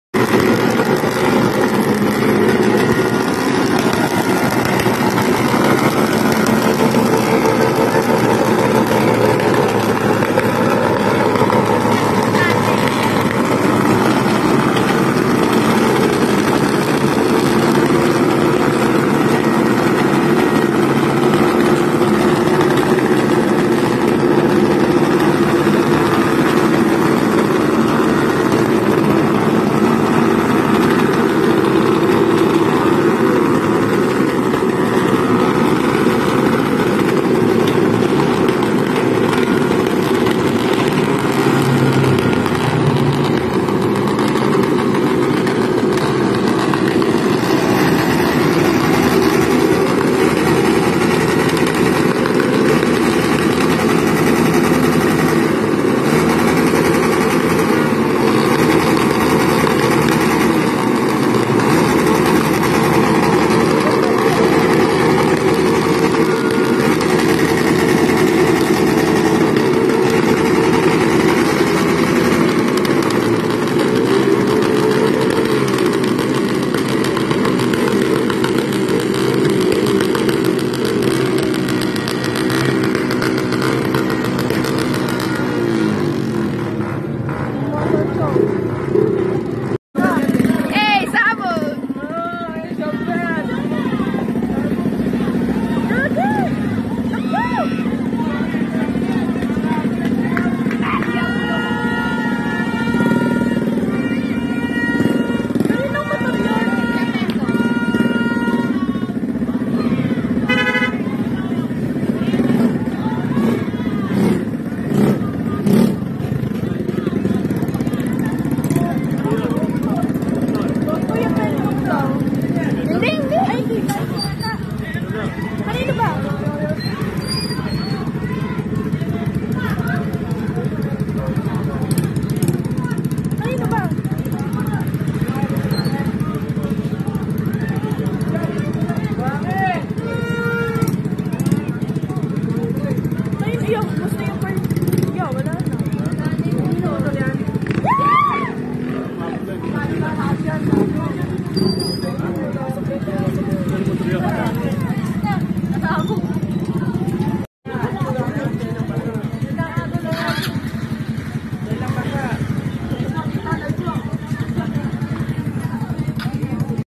Mutor nag liyab sa disperas ng bagong taon 2022/2023
Dante moto tv